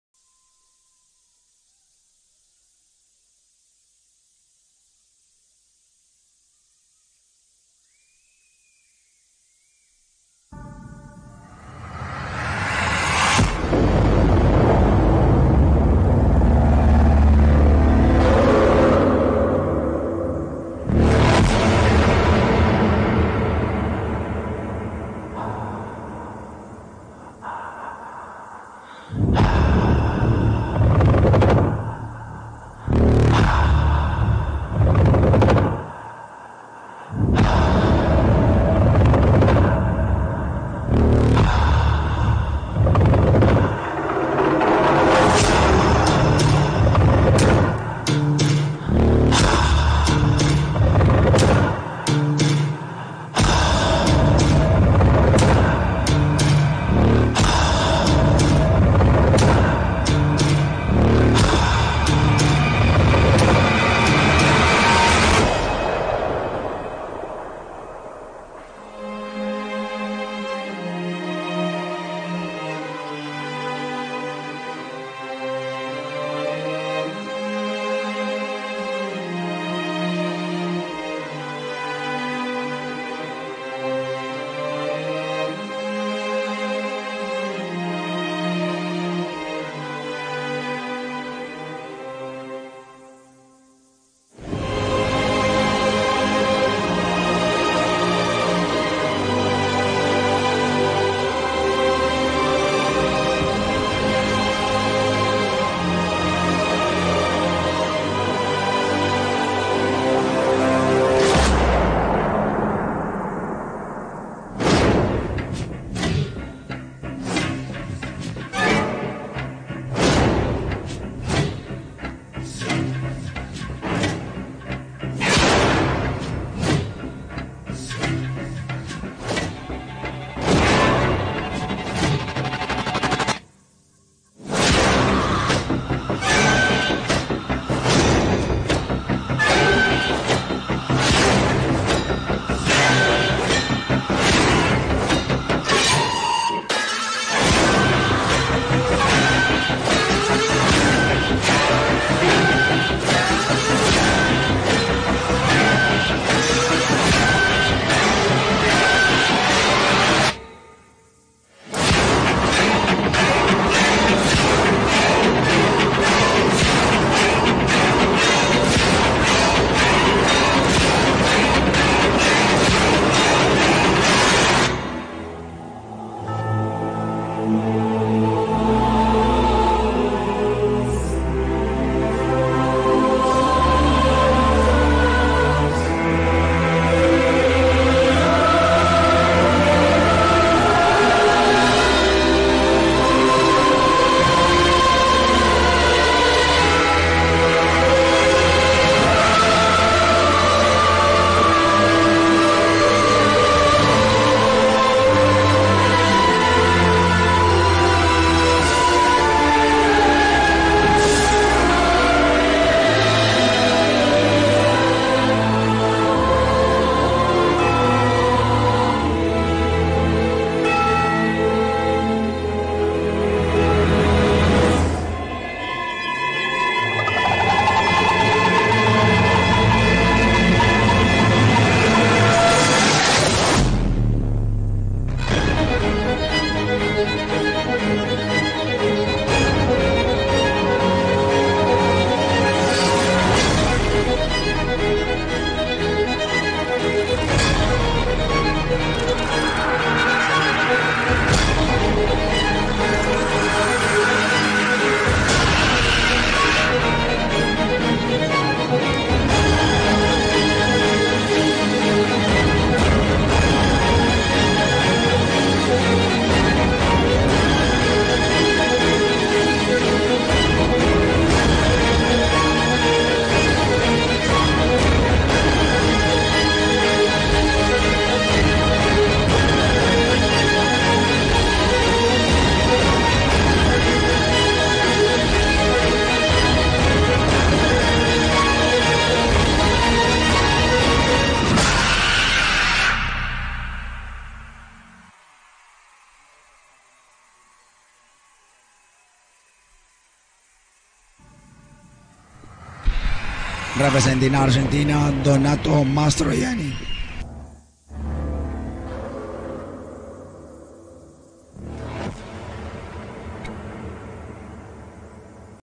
Donato Franco Mastroianni (ARG) - Long Program / Senior Men - San Juan 2023
Artistic International Series World Cup Final San Juan 2023
Patinaje artistico